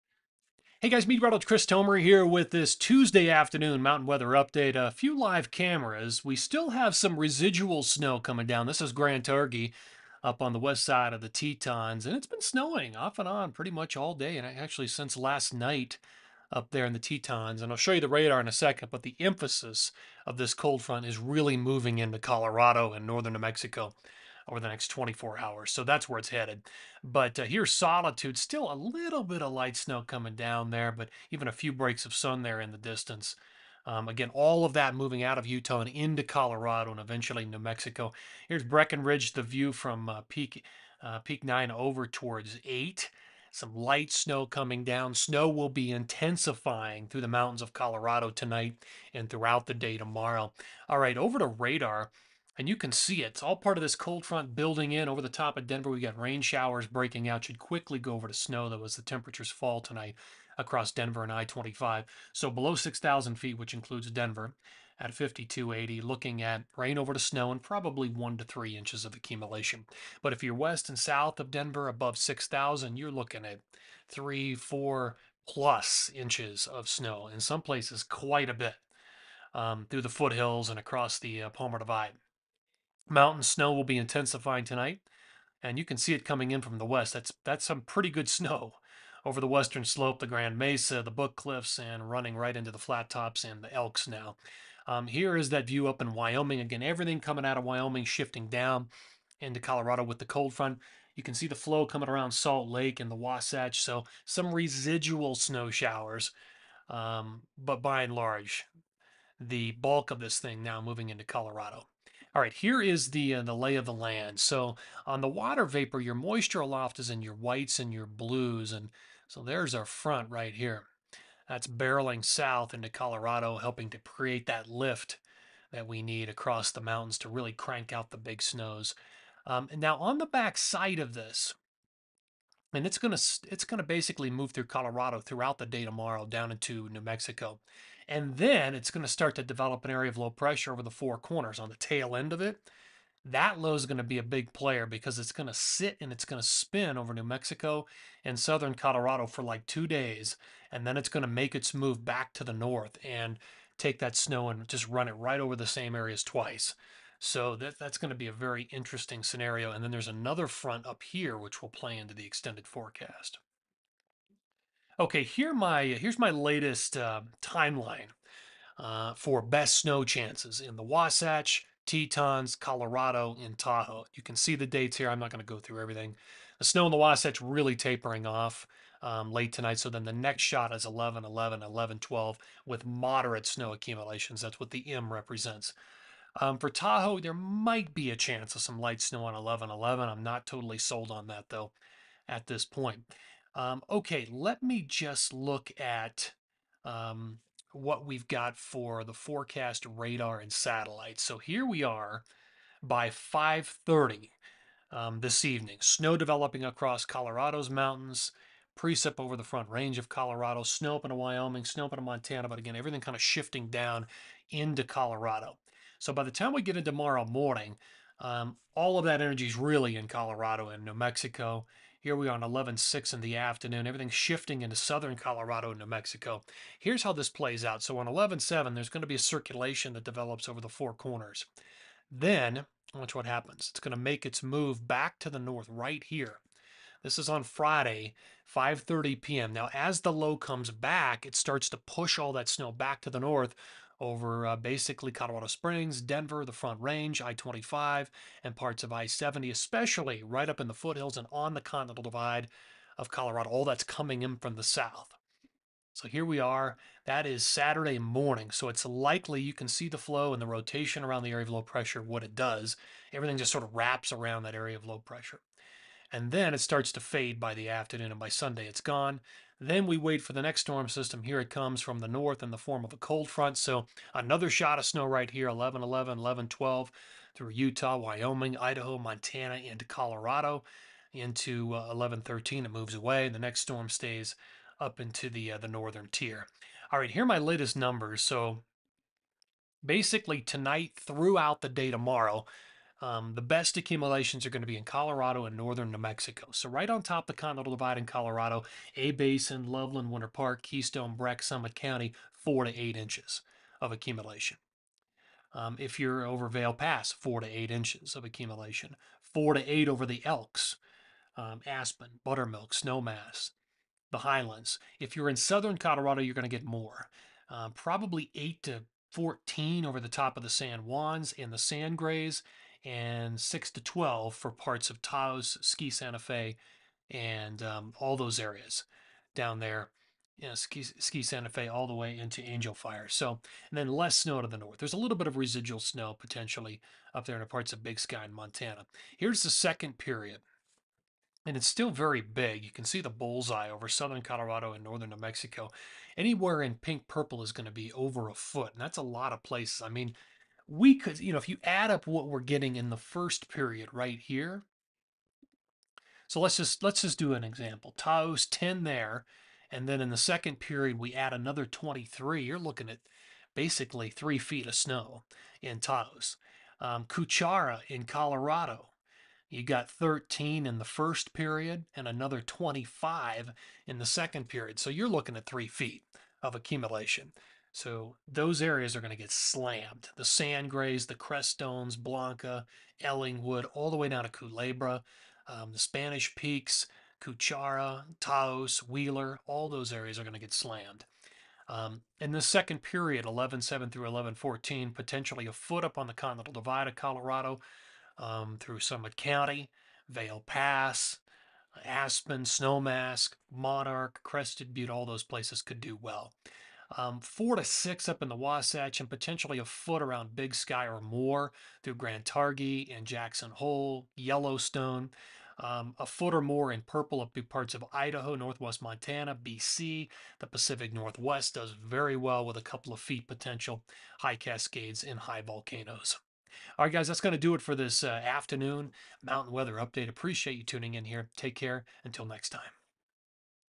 PM Mountain Weather Update 11/5, Meteorologist Chris Tomer
Snow moving out of WY & UT and into CO/NM tonight through 11/6 with moderate to heavy snow accumulation.  Cut-off low develops over NM/CO 11/7-11/9 with additional heavy snow accumulation.  Some places could see 3 feet of total accumulation (Taos, Spanish Peaks, Wheeler, Cuchara, Culebra).
Snow Timeline:
Wasatch: 11/11-11/12(M).
Tetons: 11/10-11/12(H).
Colorado: Tonight-11/6(M/H), 11/9(M/H), 11/11-11/12(M).
Tahoe: 11/11?